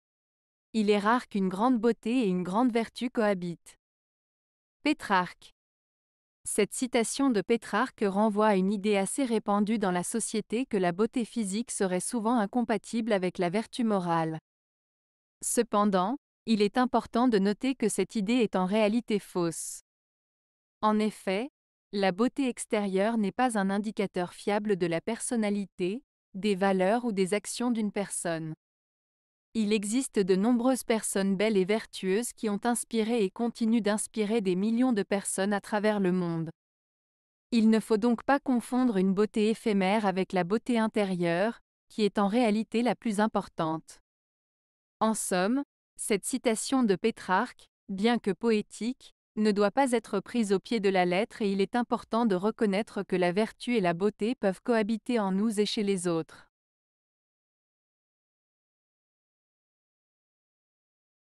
Il est rare qu'une grande beauté et une grande vertu cohabitent. - Pétrarque
Cette citation de Pétrarque renvoie à une idée assez répandue dans la société que la beauté physique serait souvent incompatible avec la vertu morale. Cependant, il est important de noter que cette idée est en réalité fausse. En effet, la beauté extérieure n'est pas un indicateur fiable de la personnalité, des valeurs ou des actions d'une personne.
Il existe de nombreuses personnes belles et vertueuses qui ont inspiré et continuent d'inspirer des millions de personnes à travers le monde. Il ne faut donc pas confondre une beauté éphémère avec la beauté intérieure, qui est en réalité la plus importante.
En somme, cette citation de Pétrarque, bien que poétique, ne doit pas être prise au pied de la lettre et il est important de reconnaître que la vertu et la beauté peuvent cohabiter en nous et chez les autres.